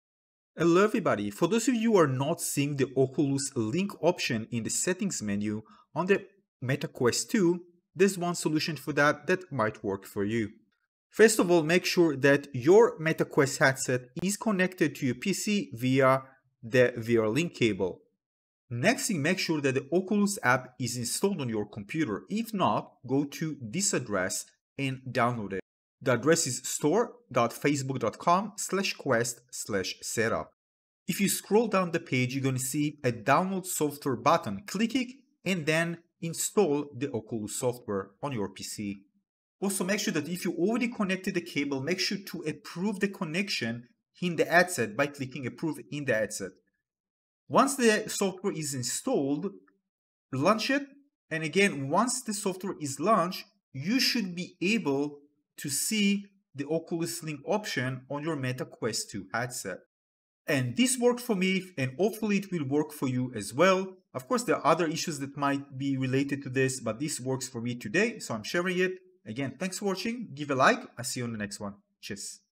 Fix: Oculus Link Option Not Showing in the Quick Settings - Meta Quest 2
A quick guide how to fix when the Oculus Link setting option is now showing on the Quick Settings menu for your Meat Quest 2 VR headset.  This solution can fix it, as it worked for me as well.

0:00 Intro
0:11 Connect Meta Quest 2 to PC & Approve Connection in the Headset
0:18 Make sure Oculus Software is Installed
0:31 Download Oculus Software
0:47 Run Oculus Software